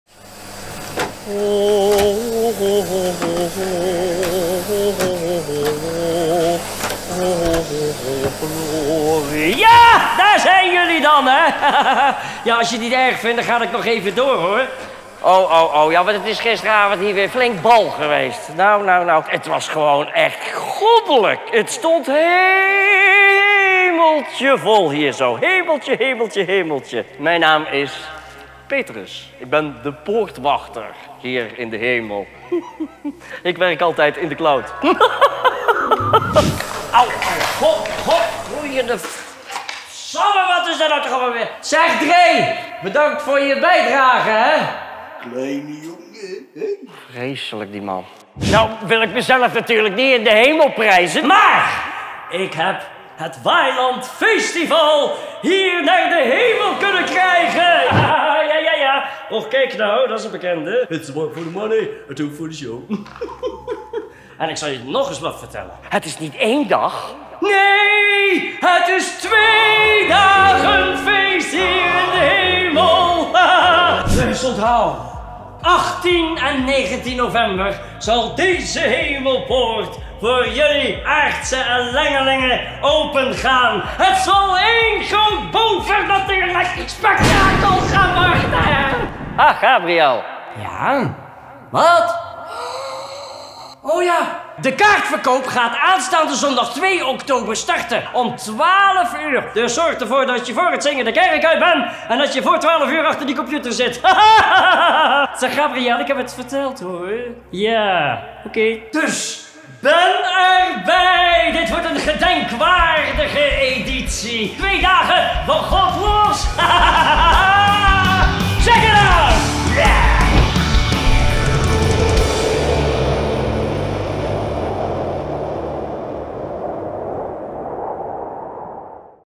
Waailand Feestival 2022 | In de Hemel - Trailer
Als donderslag bij heldere hemel wordt dit de mooiste editie van Waailand Feestival. Want Waailand Feestival vier je dit jaar niet 1, maar 2 dagen!

Dit betekent 2 dagen een goddelijke, geheime, line-up waar je u tegen zegt! 2 dagen een wonderbaarlijke show zoals je deze van Waailand gewend bent én 2 dagen een hemelsbrede mainstage waarbij stil staan geen optie is!

2 dagen uit je dak, 2 verschillende line-ups en 2 Waailand waardige shows dus!

Ben jij van god los of vier jij Waailand Feestival als een engeltje in de hemel?
18 & 19 november 2022 is er geen weg meer terug!

Tickets zijn op zondag 2 oktober, vanaf 12:00 uur, online verkrijgbaar